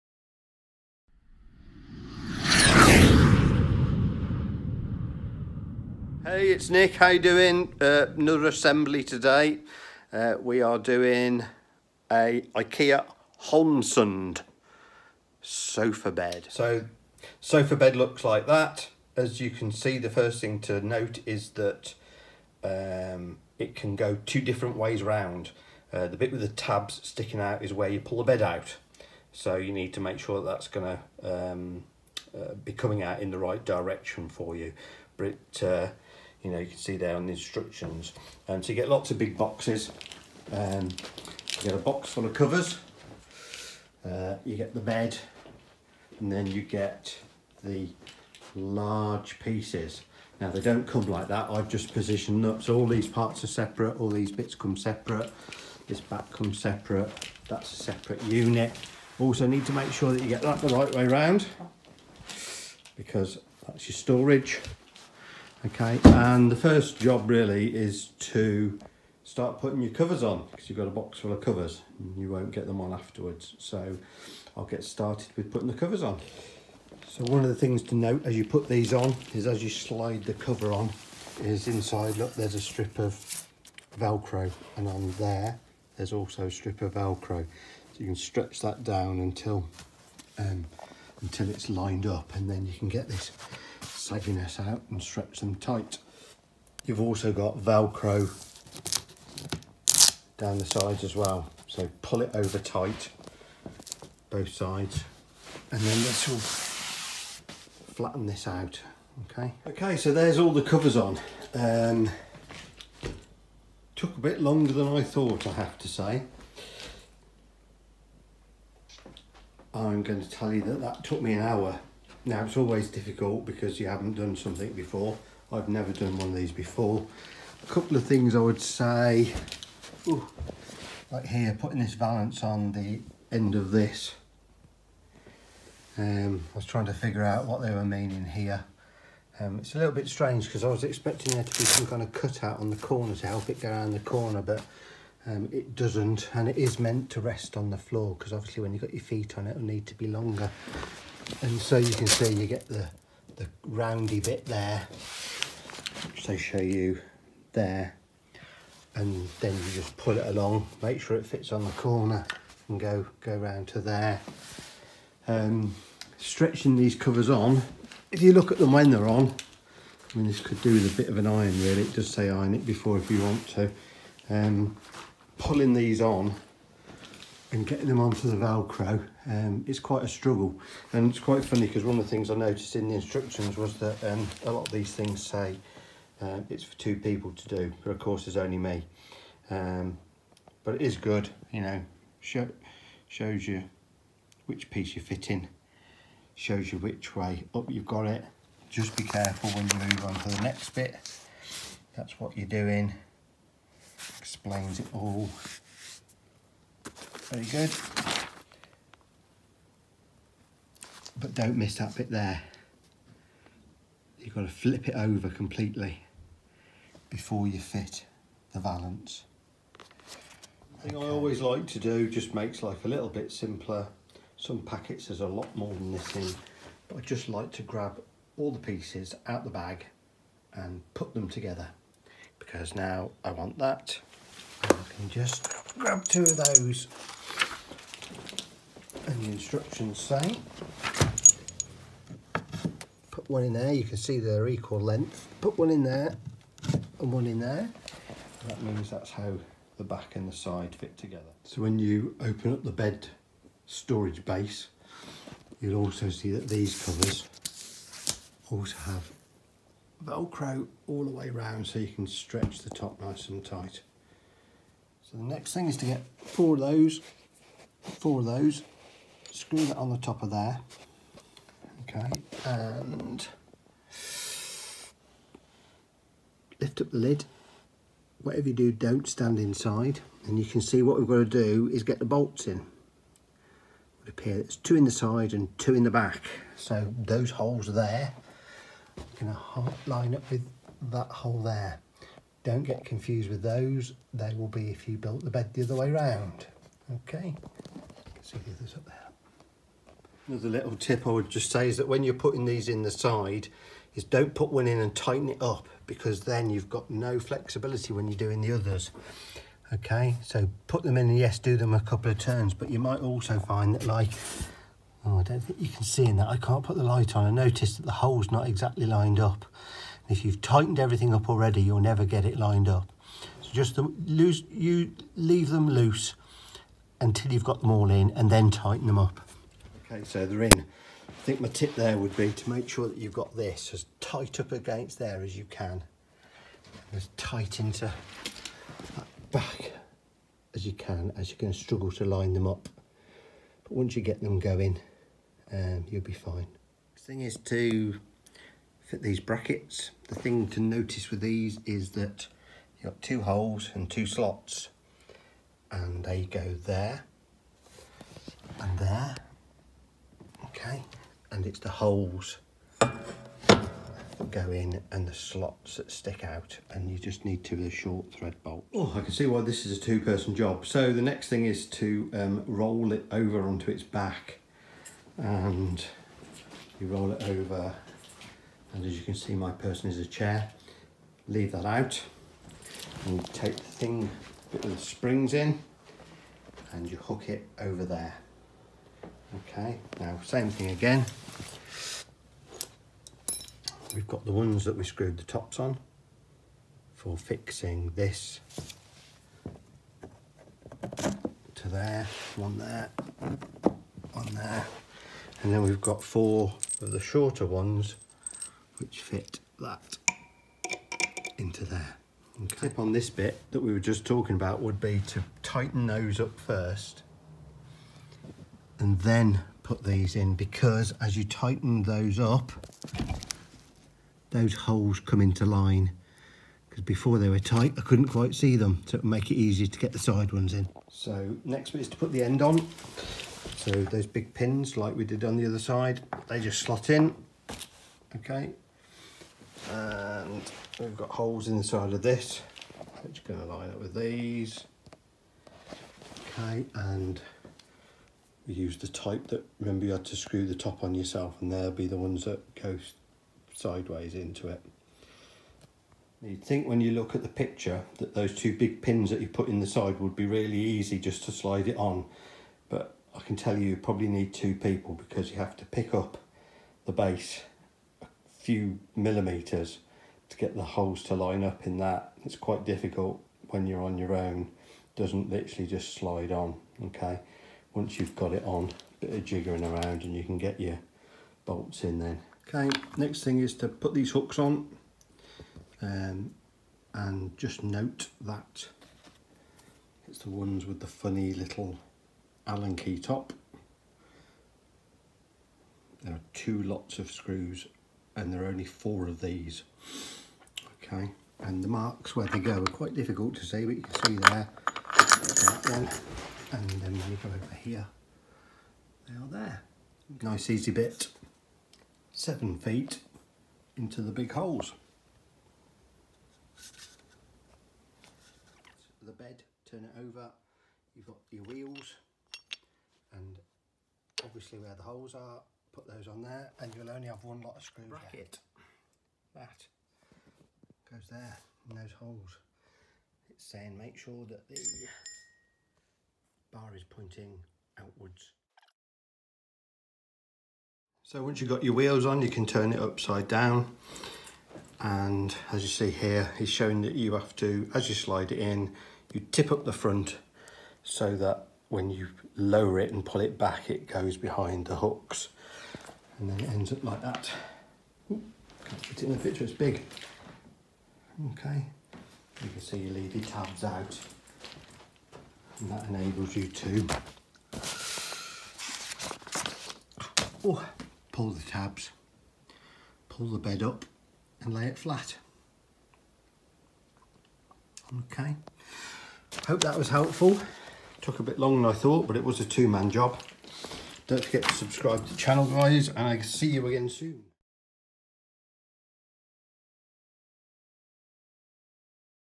IKEA Sofa Bed Assembly: A Cautionary Tale
Join me on a journey of trial and error as I attempt to assemble the IKEA Holmsund sofabed, a seemingly innocuous piece of furniture that turned out to be a daunting task. In this video, I'll share my experiences, tips, and lessons learned from following the IKEA instructions, highlighting the importance of patience and attention to detail when it comes to furniture assembly. Whether you're a seasoned DIY enthusiast or a beginner, this cautionary tale will guide you through the process of building your own IKEA sofa bed, helping you to avoid common mistakes and achieve a beautifully designed and functional home decor. With this sofa bed guide, you'll be well-equipped to tackle the challenge and create a cozy interior design that's both stylish and comfortable. So, sit back, relax, and let's dive into the world of IKEA furniture assembly!